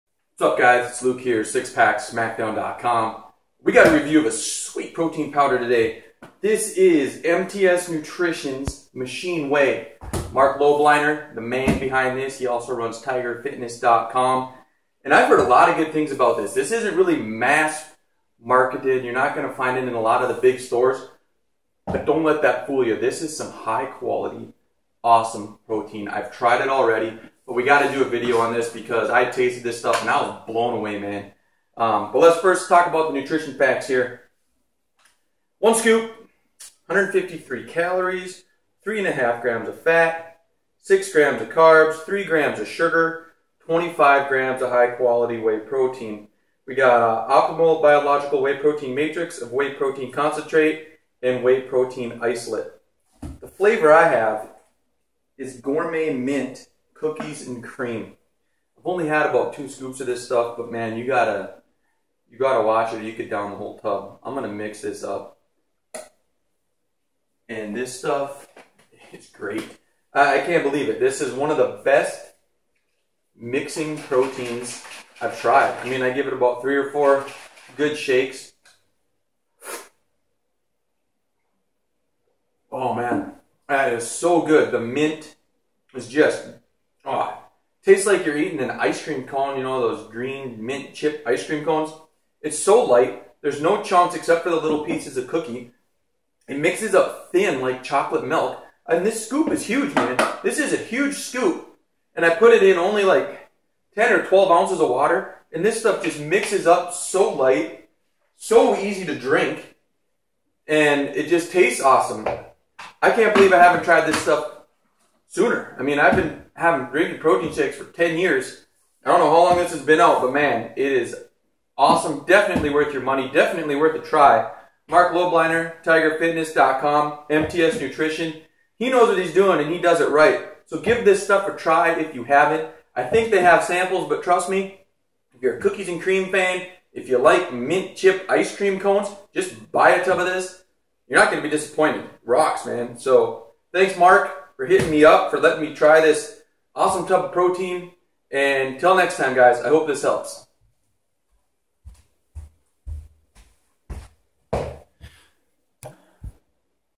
MTS Machine Whey Review - Gourmet Mint Cookies and Cream
I recently got hooked-up with some MTS Machine Whey from MTS Nutrition - the flavor was Gourmet Mint Cookies & Cream.  Big thanks to Marc Lobliner for hooking me up and I have to say, this is one of the best tasting and best mixing proteins I have ever tried!  The scoop size was pretty big so I was expecting my shake to be rather thick and maybe chunky, but this was NOT the case.  MTS Machine whey mixed up so light and smooth it was almost like drinking mint-flavored chocolate milk.

After having tried probably 50 to 100 different protein powders, MTS whey definitely stands out in my  mind and would probably be ranked in my top 5 powders that I have ever tried.  You don't even need a sample of this stuff.  If you like chocolate protein or cookies and cream protein powders, you can just throw-down for a 5 pound tub (or buy 10 lbs because you'll be drinking this stuff like it's candy).

MTS Machine Whey Review Highlights

Mixed up light and smooth - not a single chunk - it was awesome
Tasted REALLY GOOD 10/10
Nutrition profile is solid - 25g protein, 3.5g fat, 153 calories
Recommendation:  DEFINITE BUY